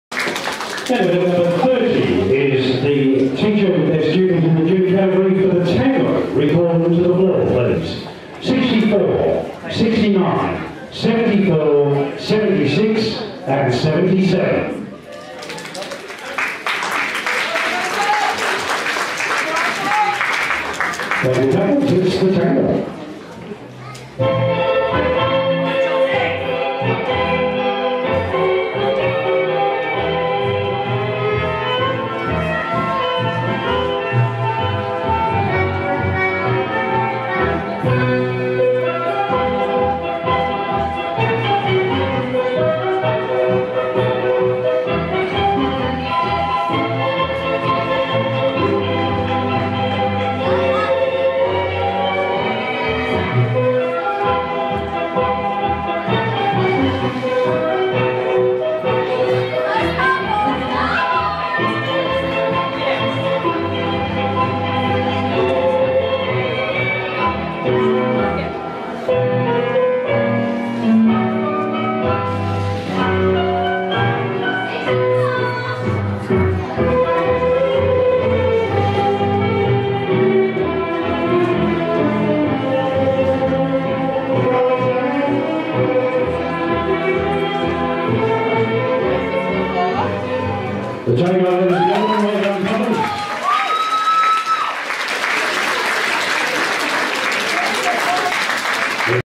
030 Teacher Student Junior Std Tango Final
Video from the 2020 Perth Ballroom Challenge competition on 20/9/2020.